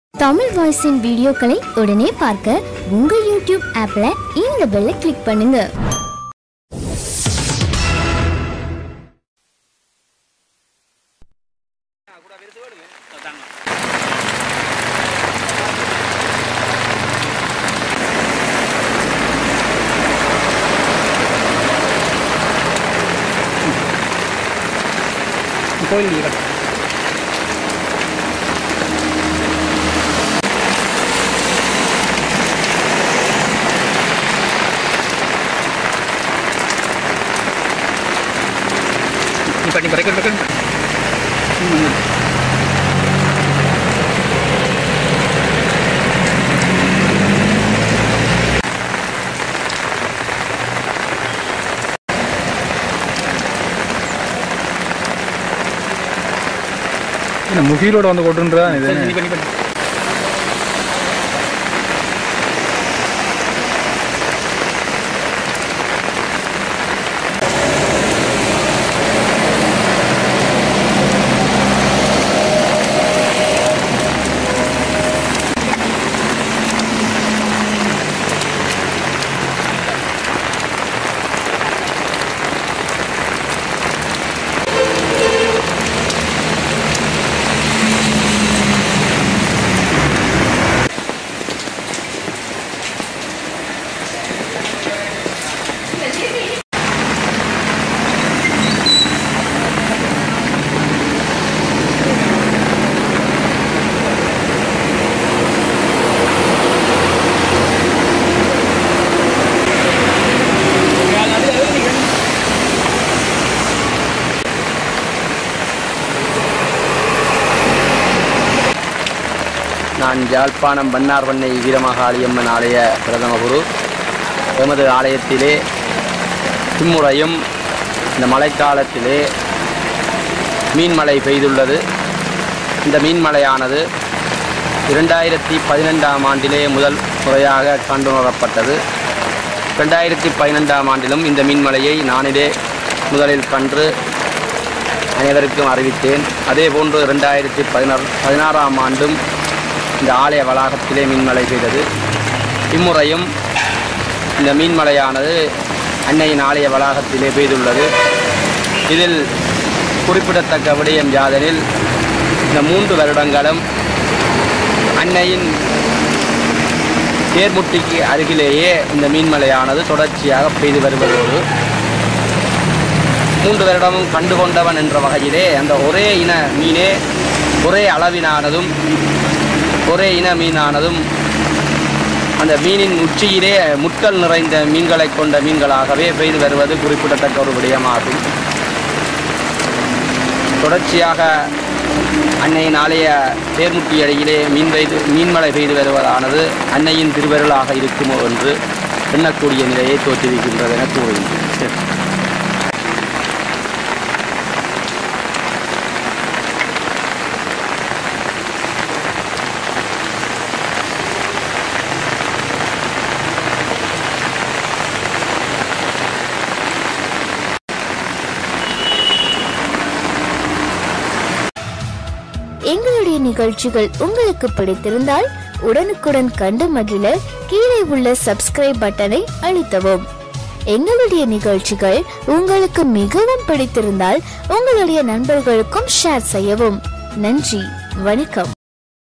நல்லூரில் 'மீன் மழை'... வியப்பில் மக்கள்! - Fish rain down from Sky - Tamil Voice
Fish rain down from Sky
நல்லூரில் 'மீன் மழை' வியப்பில் மக்கள்!

யாழ்ப்பாணம் நல்லூர் வீரமாகாளி அம்மன் ஆலயப் பகுதியில் தற்போது 'மீன் மழை' பெய்துவருகிறது.

மழையுடன் சில வகை மீன்களும் நிலத்தில் பரவலாக வந்து வீழ்கின்றன.

இந்த நிகழ்வு தொடர்ச்சியாக மூன்றாவது ஆண்டாக நடைபெறுவதாக அந்தப் பகுதியைச் சேர்ந்த பெரியவர்கள் தெரிவித்தனர்.

அந்தப் பகுதியில் பெருமளவானோர்கூடி ஆச்சரியமாகப் பார்வையிடுகின்றனர்.